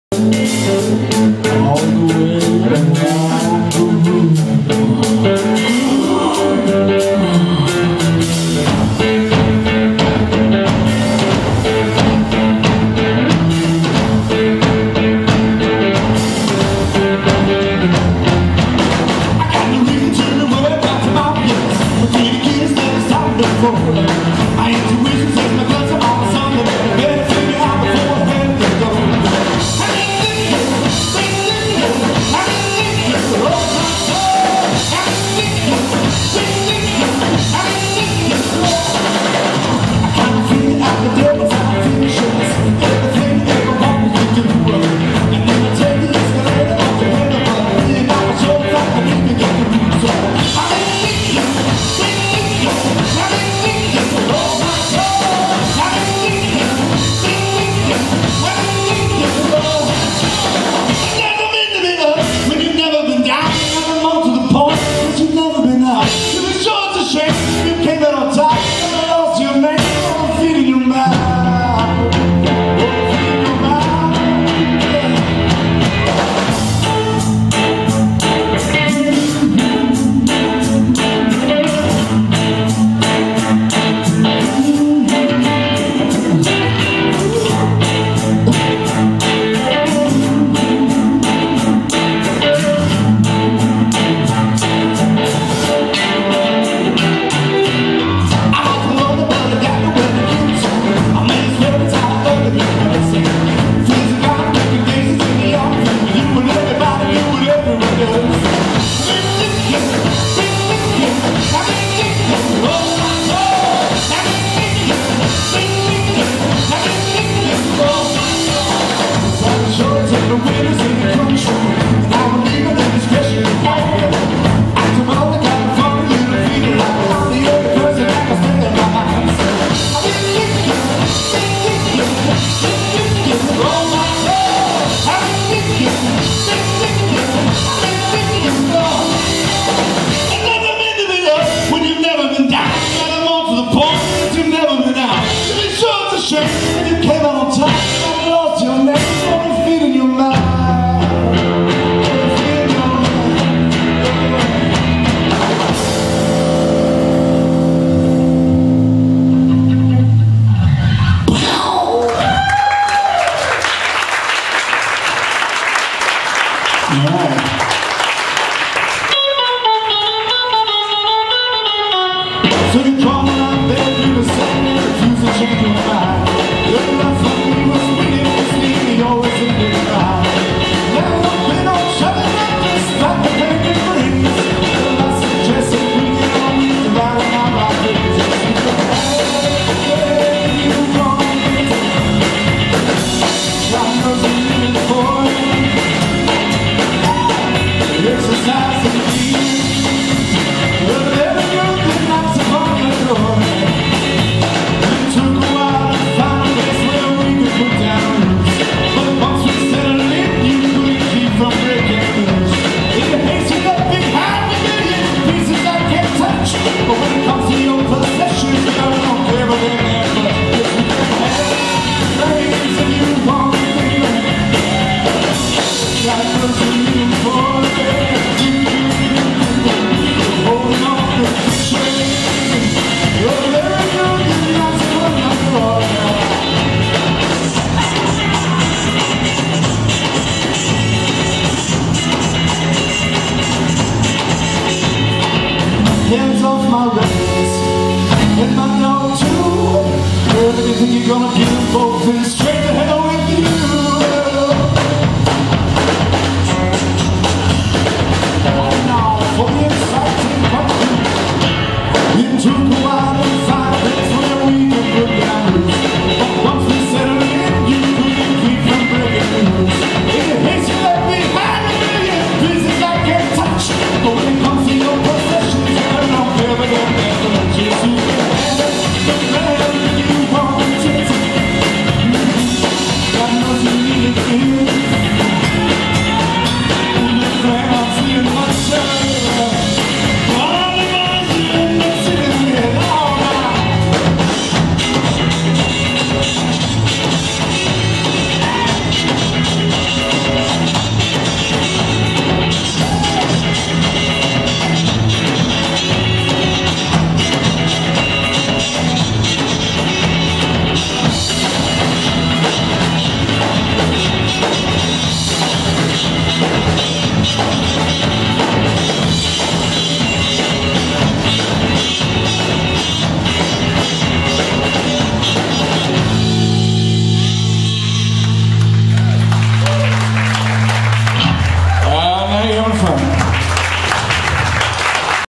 Oil Boom at The Kessler Theater in Dallas, Texas
Recorded live on July 22, 2011 at the historic Kessler Theater in X+ North Oak Cliff. Also on the bill were Hunter Hendrickson and Kirk Thurmond.